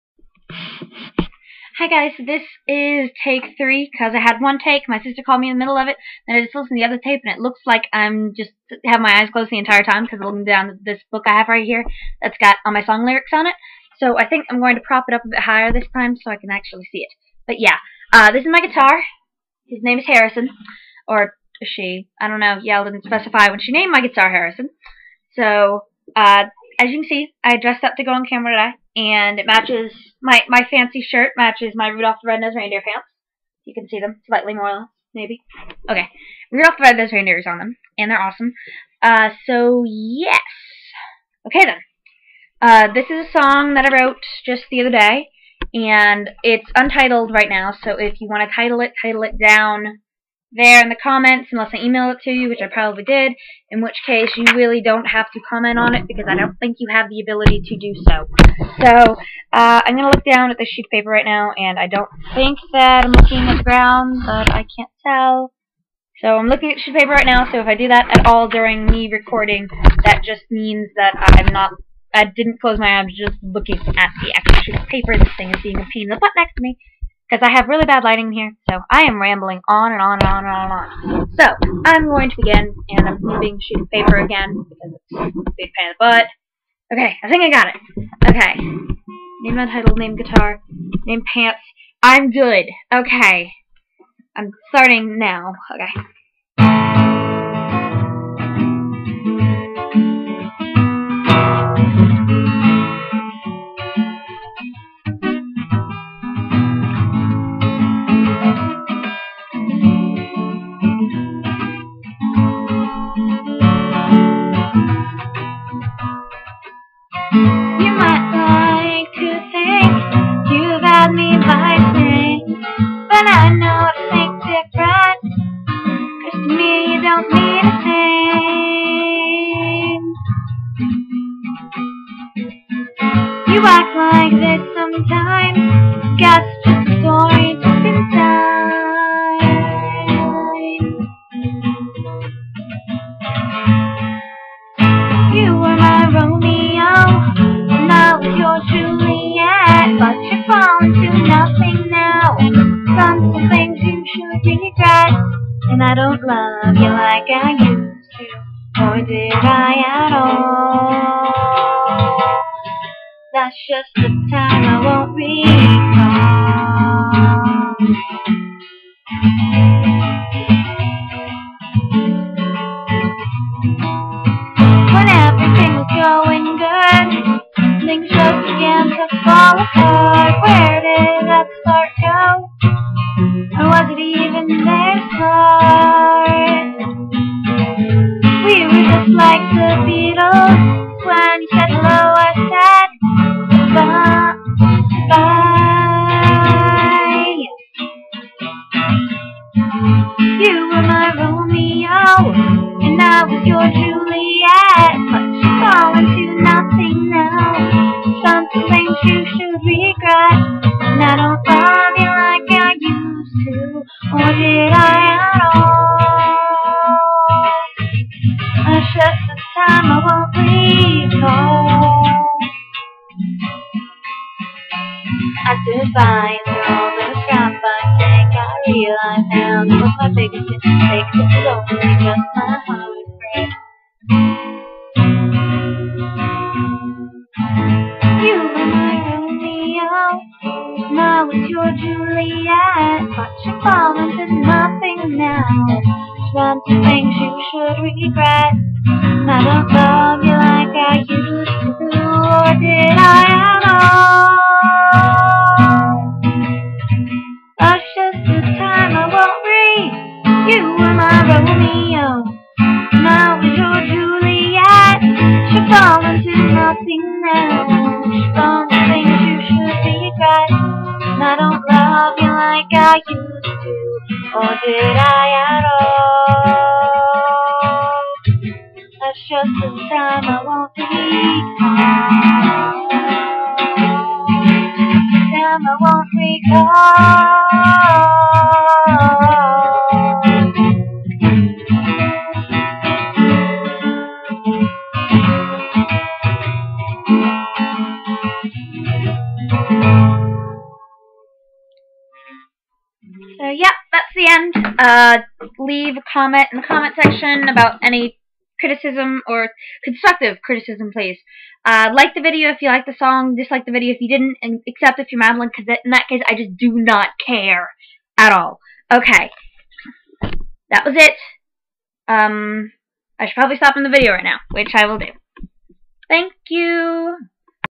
un titled song - Kelsey W.
Hi guys! This is take 3 of my video, let me know if you want take 4 which is slightly worse than this one. This is the first song i've ever written, so give me some positive feedback or constructive critisism. Open critisism will be deleted. and who i talk about at the end, the person that I don't care what you think, you know who you are. I am not just insulting everyone with that name.